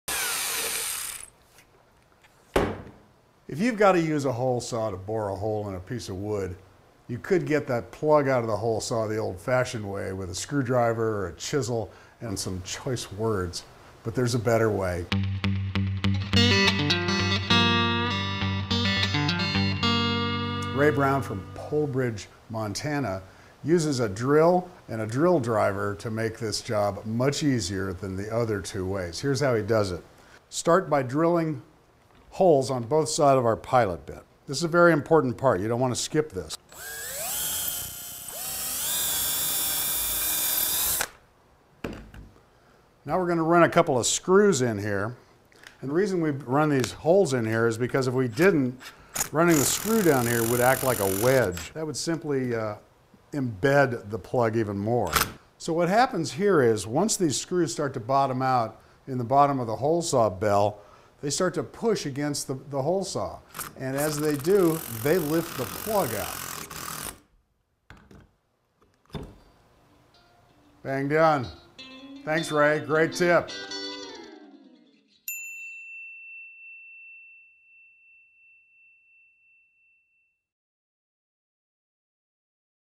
How to Remove Wood Plugs from Hole Saw
When you need to use a hole saw to bore a hole in a piece of wood, you could get that plug out with a screwdriver and some choice words, but there's a better way. Fine Homebuilding's Chuck Miller shows you how!